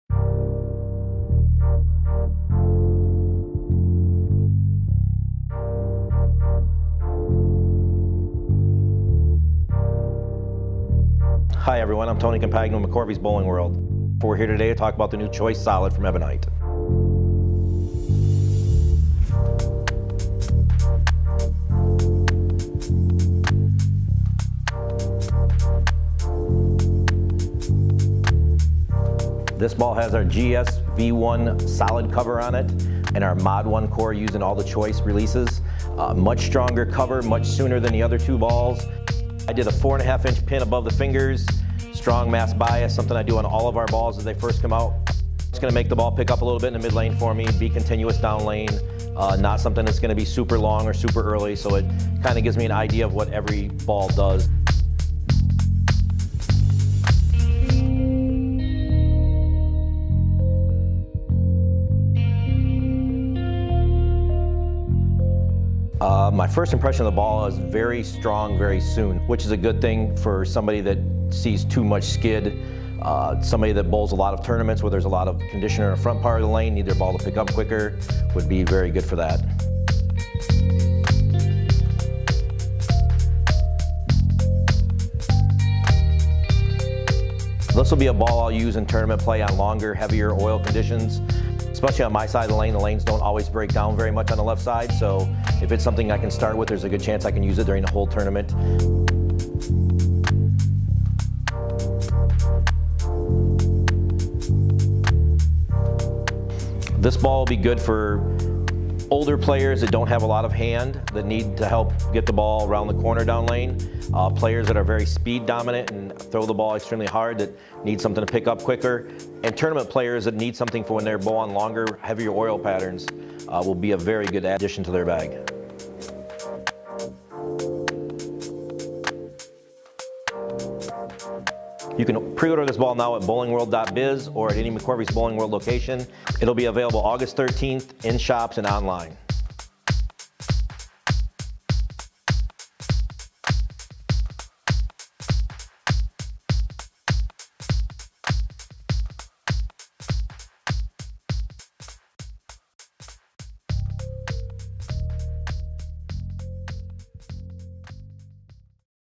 Ebonite Choice Solid - Ball Review Video
Introducing the new Ebonite Choice Solid, featuring the GSV-1 Solid cover wrapped around the MOD-1 asymmetric core. This ball is available to the public 8/13/19

Special thanks to Ebonite Bowling and Ten Pen Strike & Spare (Louisville, Ky) for making this video possible!